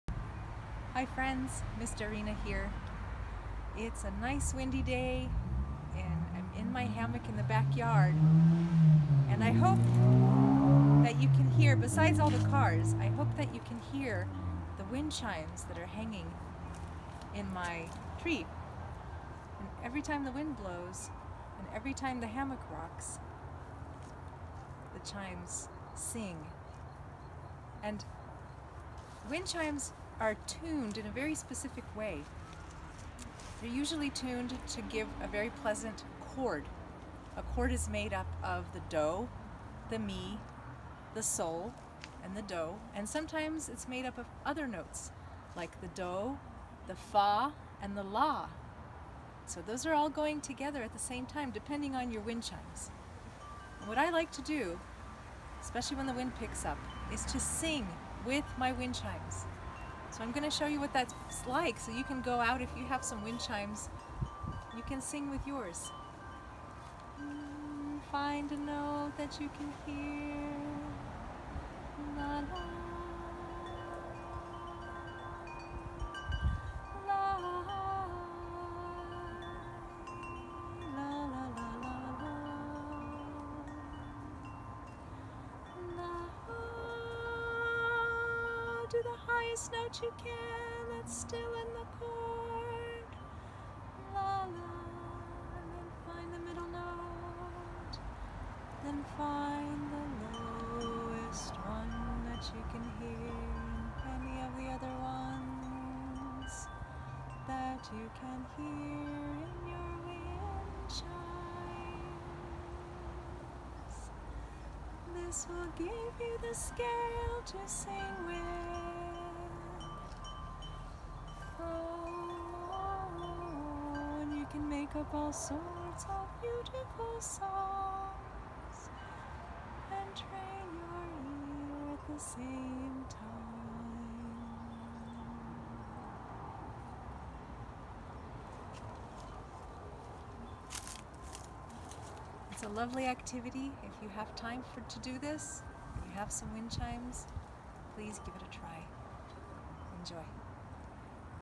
Harmonizing with Wind Chimes
A fun thing to do in the hammock with singing, and ear training